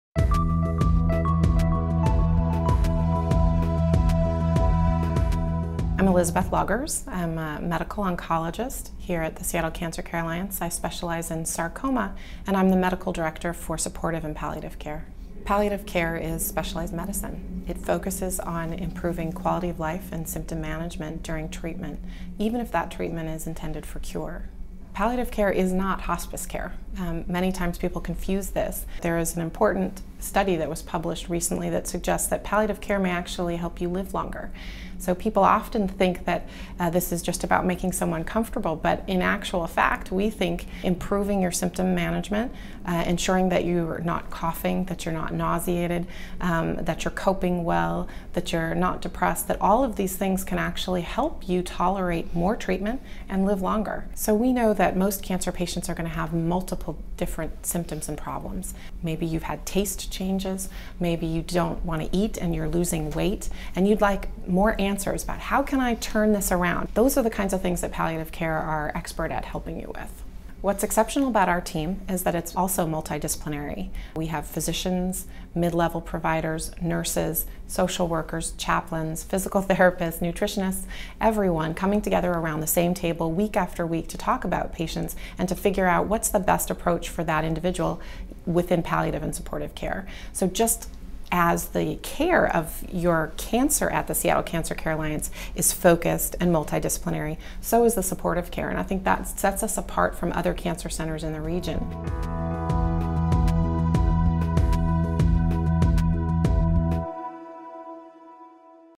Palliative Care at Seattle Cancer Care Alliance | Dr. Elizabeth Loggers
Dr. Elizabeth Loggers describes the palliative care program at Seattle Cancer Care Alliance. Palliative care is specialized medicine that focuses on improving quality of life and symptom management during treatment. Many people palliative care with hospice care, but that would be a mistake. An important study suggests that palliative can make people live longer by improving care, symptom management, and physical and emotional well-being. SCCA offers a multidisciplinary team that includes doctors, mid-level providers, nurses, chaplains, physical therapists and nutritionists that work together to provide the best care. Dr. Loggers is a medical oncologist at Seattle Cancer Care Alliance who specializes in treating patients with sarcoma and the medical director for Palliative and Supportive Care.